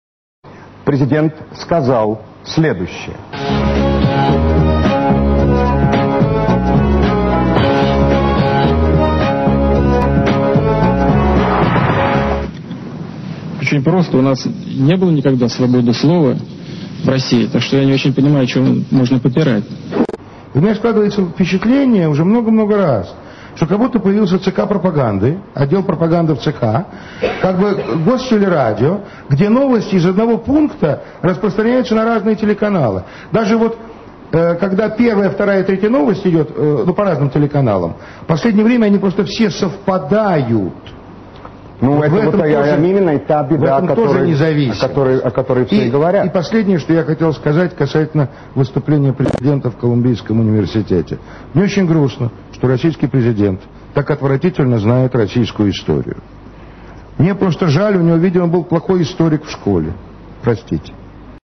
Несвободный НТВ сказал что Черкизов плохо учился
НЕ СВОБОДНЫЙ телевизор свободно и негативно обсуждает слова Путина. Дяденька Черкизов, в несвободном телевизоре говорит, что ему хочется слышать много разнообразной лжи по поводу одного и того же события. Казачок из госдепа?
Зажим демократии и свободы слова!

Мое мнение: В. Путин сказал абсолютно правильно! России не нужна госдеповская дерьмократия и абсолютная и безответственная "свобода слова", разрушающая страну и превращающая сознательных граждан в предателей собственной страны.

Абсолютная свобода слова - есть предательство Родины. Каждый гражданин страны должен отвечать за свои слова. Особенно, если он распространяет ложь, подрывает власть и порочит страну!